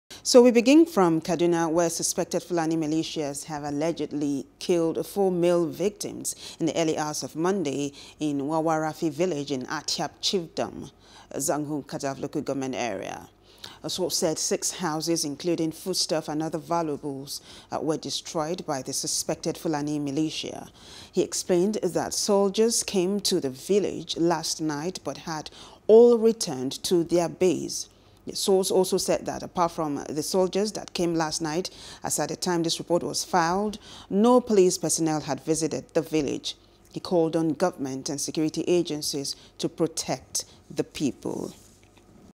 Southern Kaduna: Four People Killed, Houses Destroyed By Militia | NEWS
Suspected Fulani militias have allegedly killed four male victims in the early hours of Monday in Wawarafi village in Atyap Chiefdom, Zangon Kataf local government area, Kaduna State.

A source said six houses, including foodstuff and other valuables, were destroyed by the suspected Fulani militia.